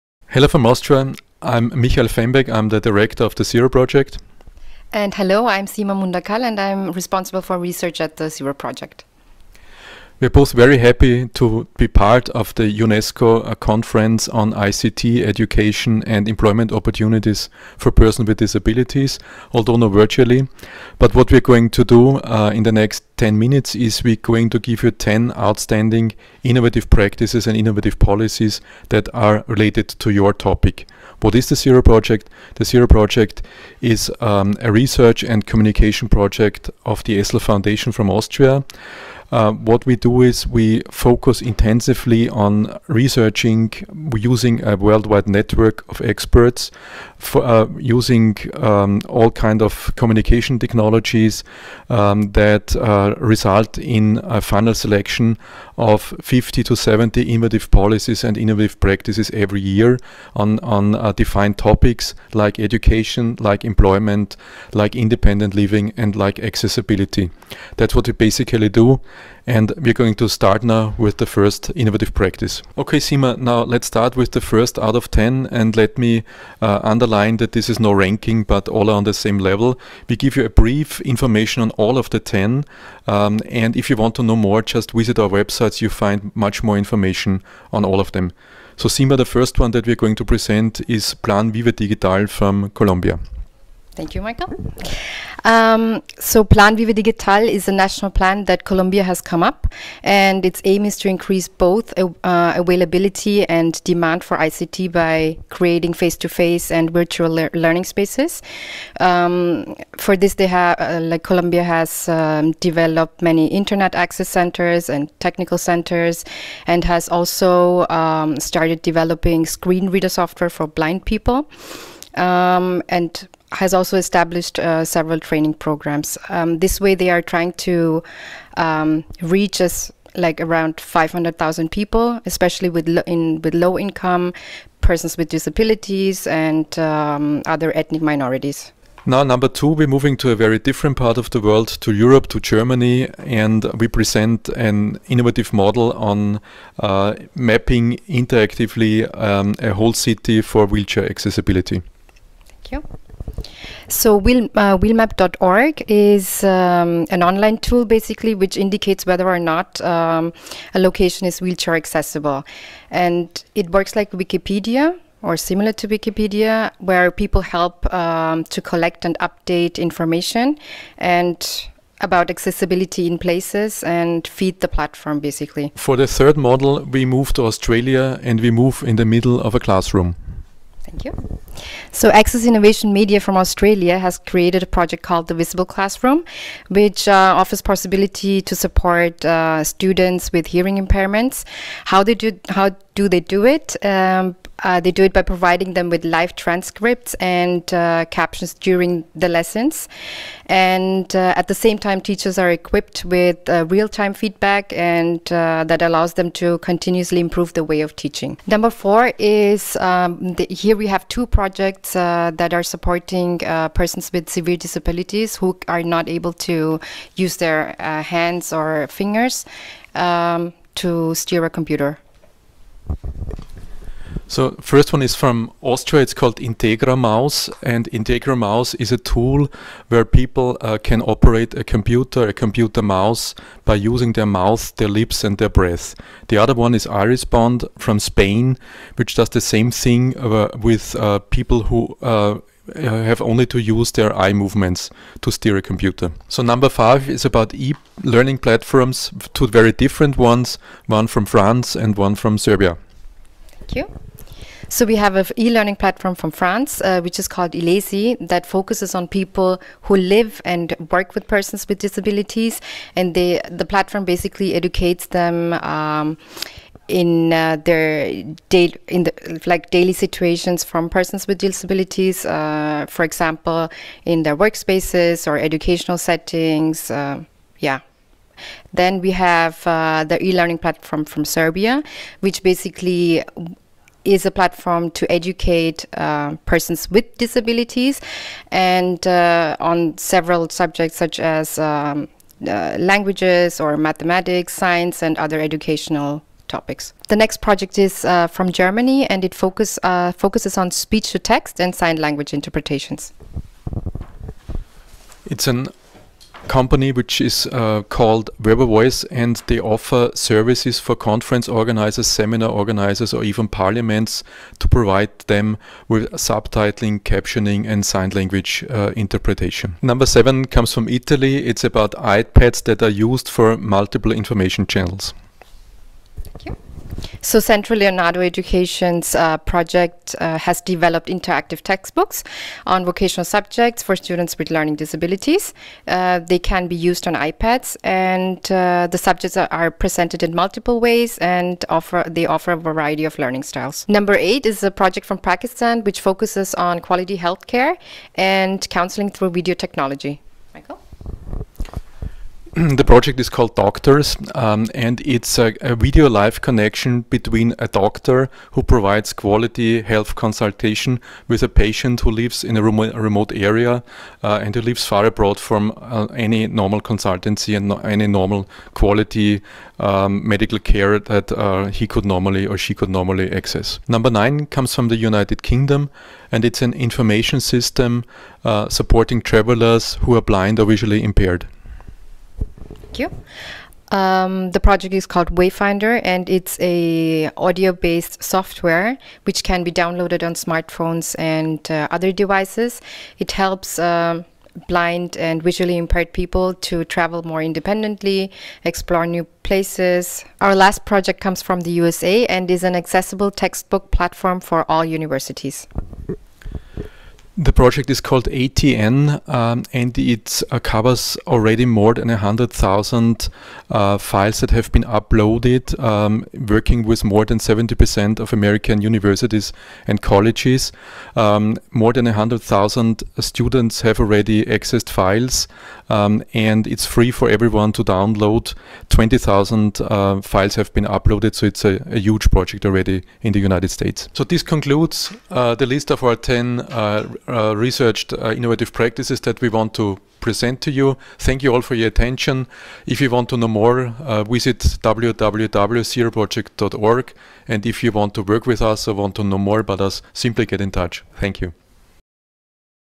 Zero Project Message to the UNESCO Conference on ICTs for Persons with Disabilities
This is a message to the UNESCO Conference on ICTs for Persons with Disabilities in Costa Rica from 14th - 16th November 2016.
Michael Fembek, Director of Zero Project & Seema Mundackal, Research present 10 Innovative Practices.